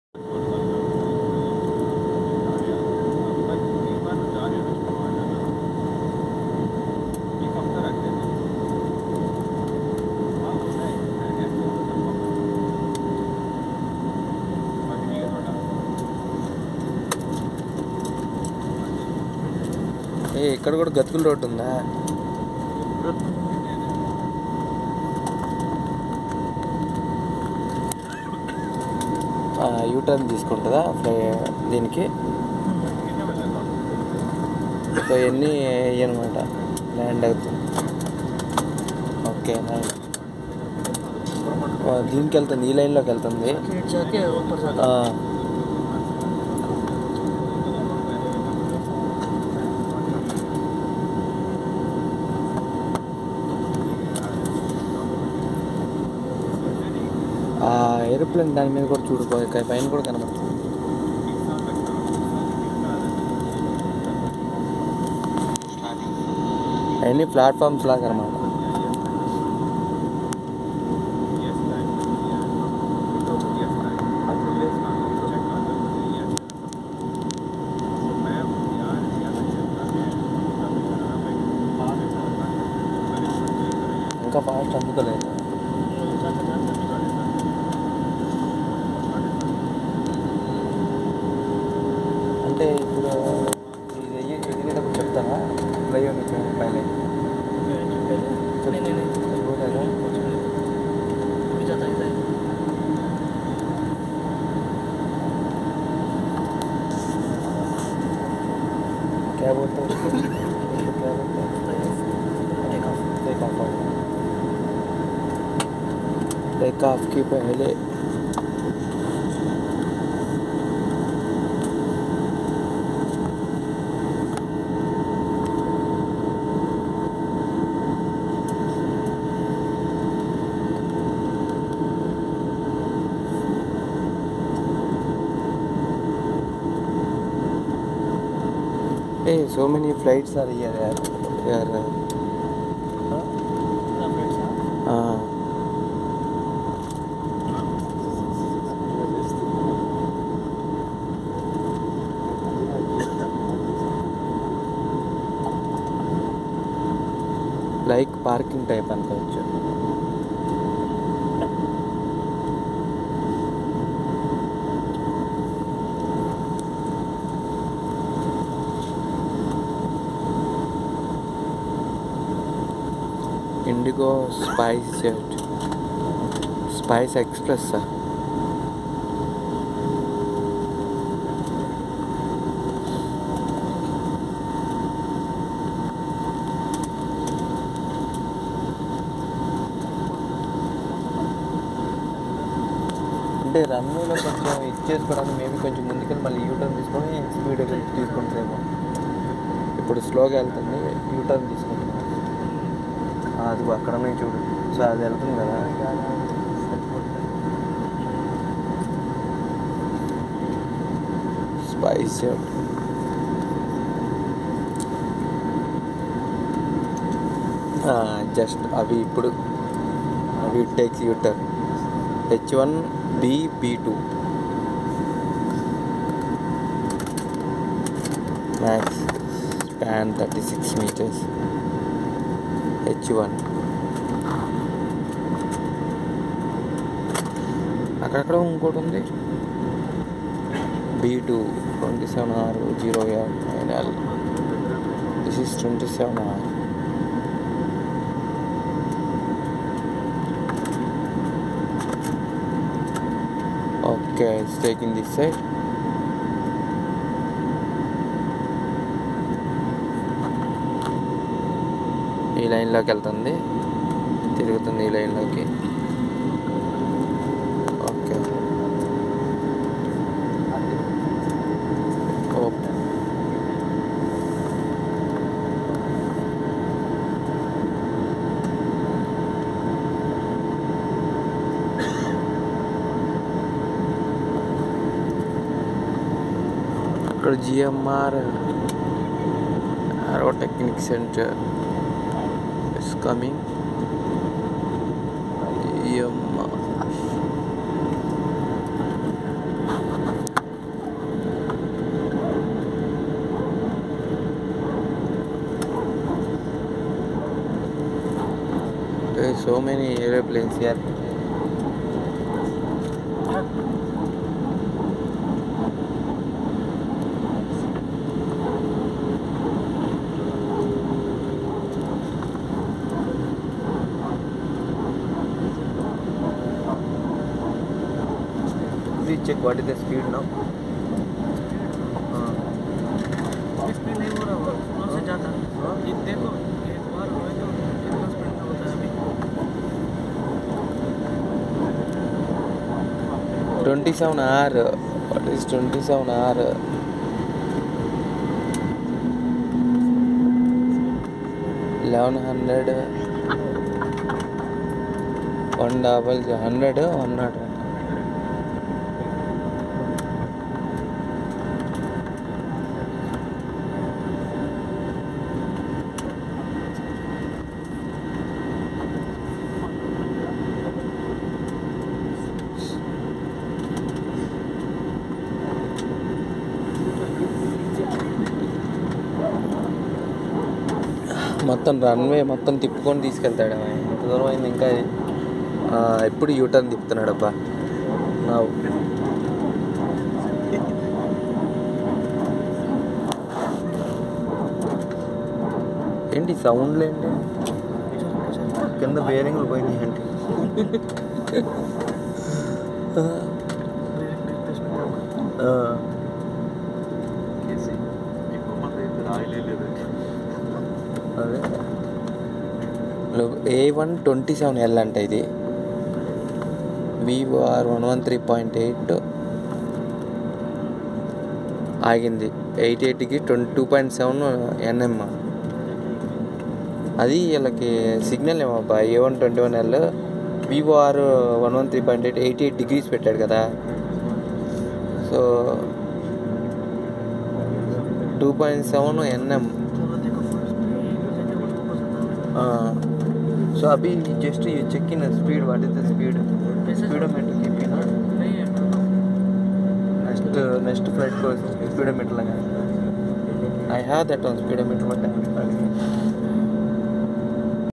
Detail view of Hyderabad Airport RUNWAY before going to take take-off
Hyderabad airport runway before going to take off